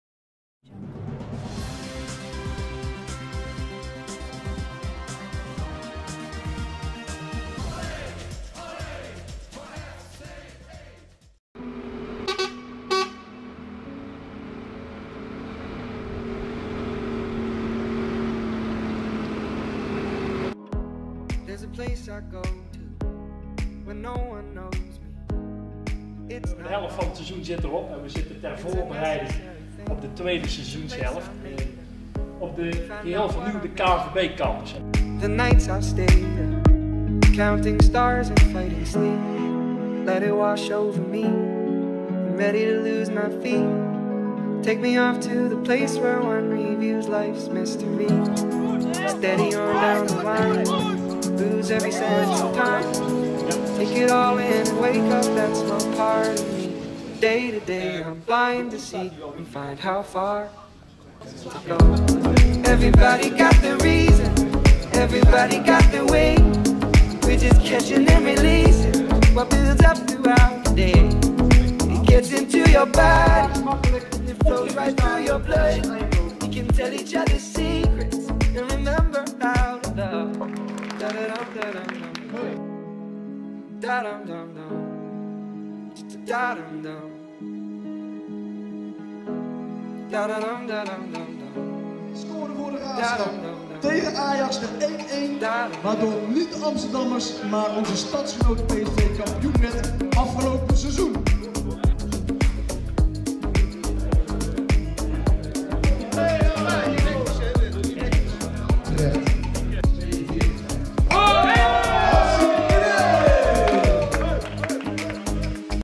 Trainingskamp dag 2: Uniek FCE Connect-event tijdens trainingskamp
Gedurende de tweede dag van het trainingskamp op de KNVB Campus in Zeist kwam er een groep sponsoren op bezoek bij FC Eindhoven. Zij kregen een uniek kijkje op de campus en bij het trainingskamp!